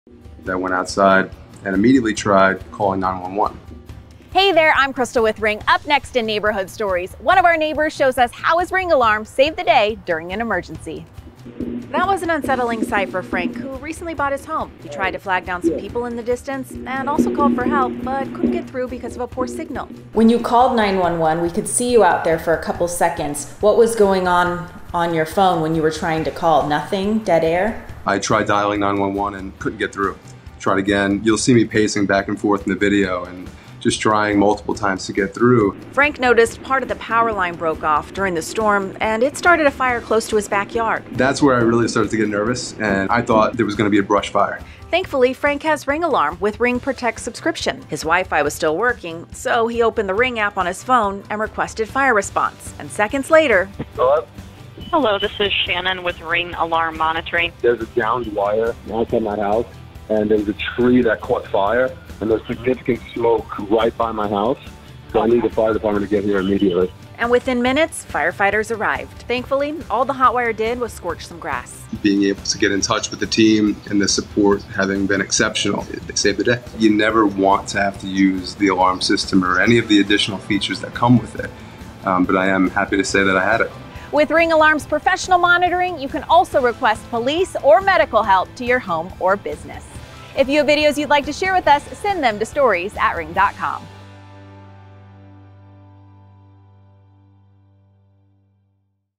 “I Need The Fire Department To Get Here Immediately!” | Neighborhood Stories
In this week’s Neighborhood Stories: Frank showed us how Ring Alarm helped him when he couldn’t get through to 911 during an emergency.